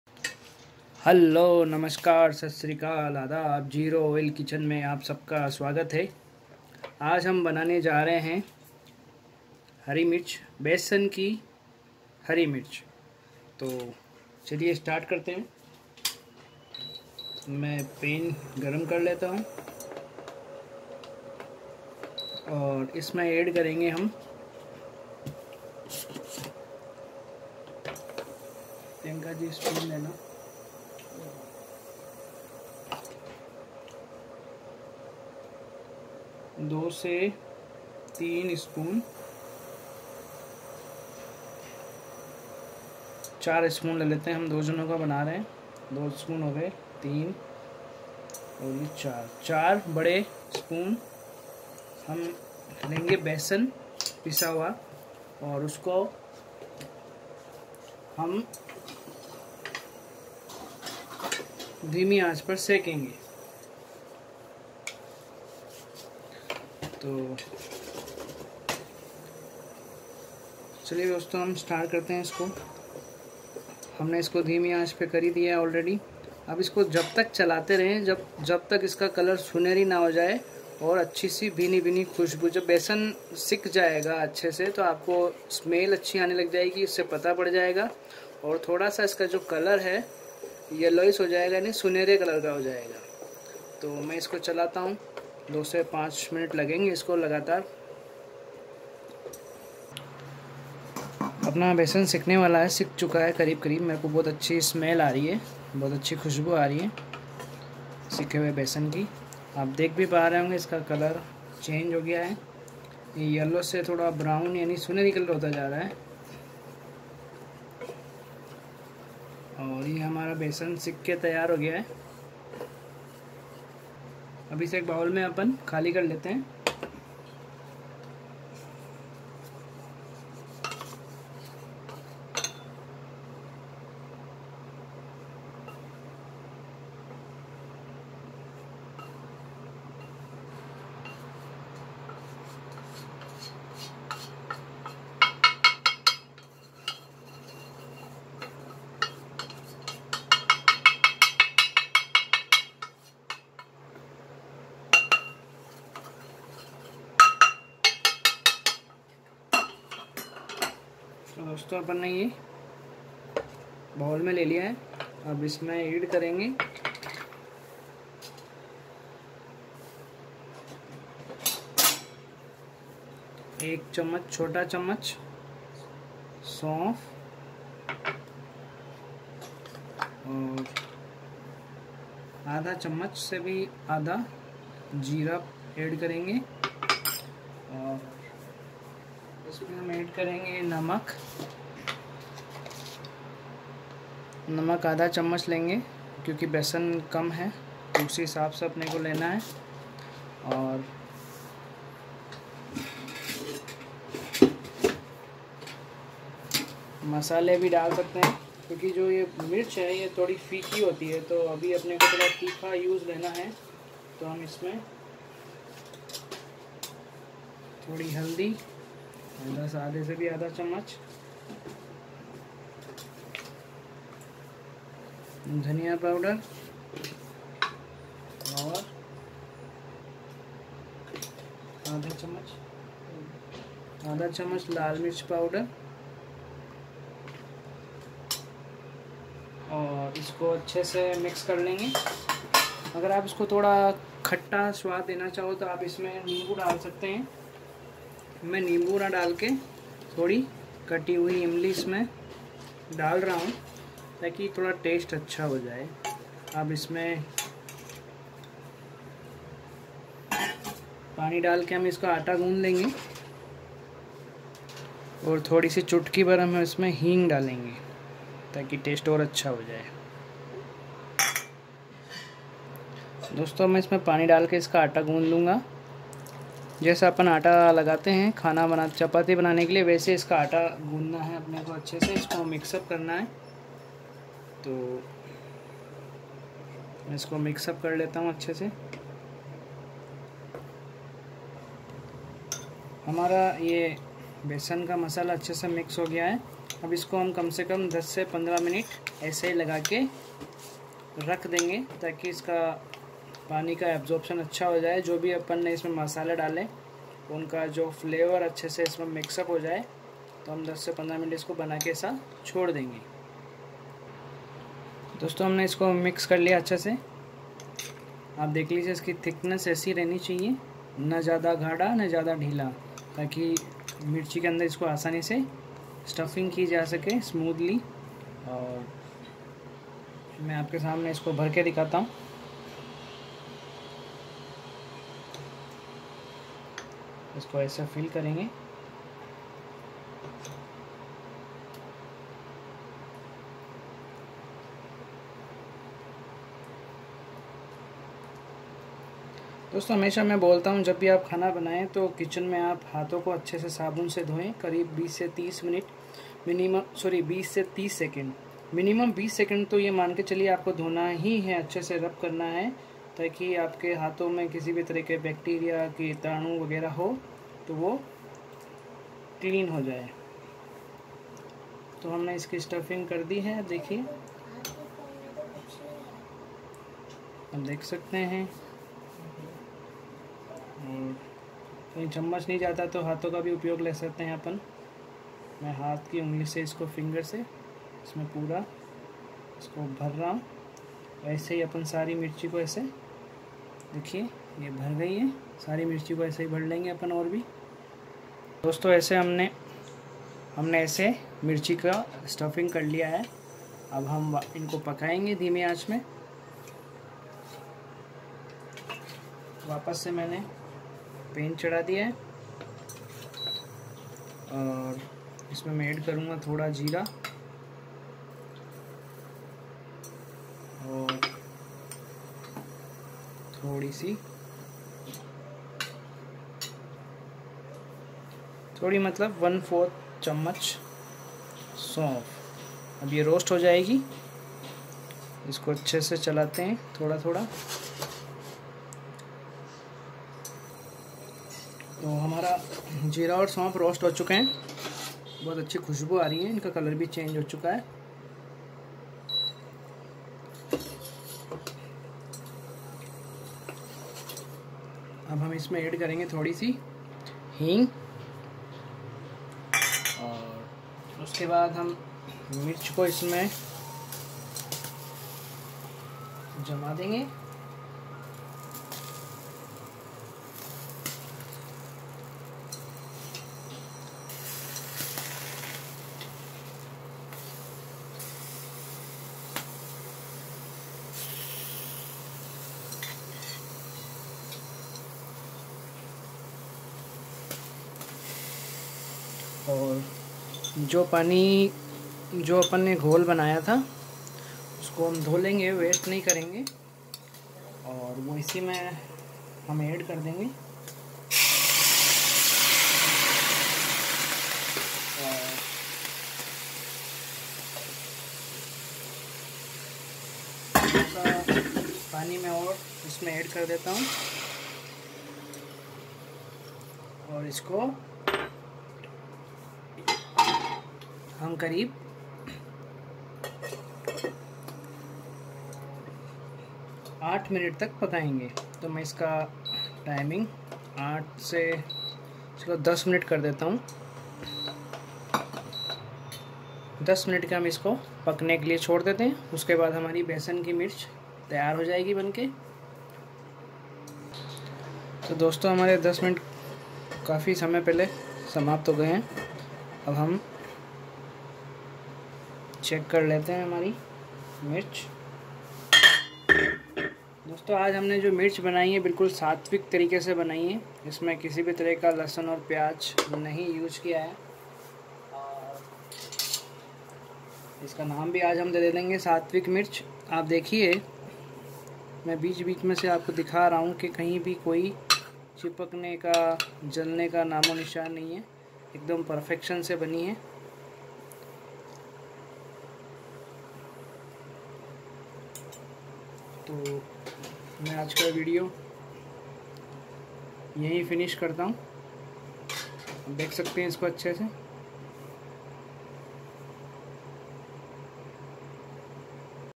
Satvik Baisan Mirch Oil Free II सात्विक बेसन मिर्च आॅयल फ्री
सात्विक बेसन मिर्च आॅयल फ्री
प्रयुक्त सामग्री:-
40 gm बेसन
4-5 हरी मिर्च
1 Tsp सौंफ
1 Tsp जीरा
1/2 Tsp लाल मिर्च पाउडर
1/4 Tsp हल्दी पाउडर
1/2 Tsp धनिया पाउडर
1/2 Tsp इमली/ 1/2 नींबू
चुटकी भर हिंग
नमक स्वादानुसार